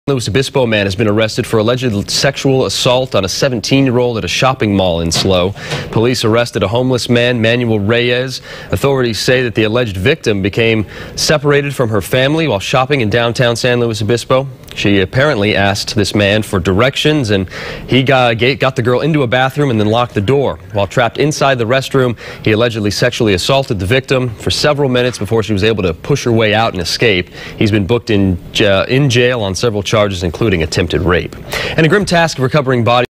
San Luis Obispo Arrest for Sexual Assault
A SAN LUIS OBISPO MAN HAS BEEN ARRESTED FOR AN ALLEGED SEXUAL ASSAULT ON A 17-YEAR-OLD AT A SHOPPING MALL.